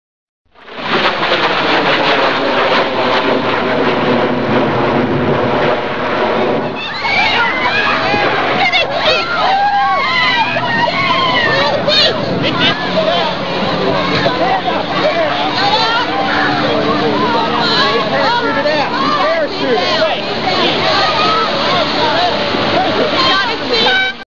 F-117 Airshow crash
An amateur video of a F-117 crashing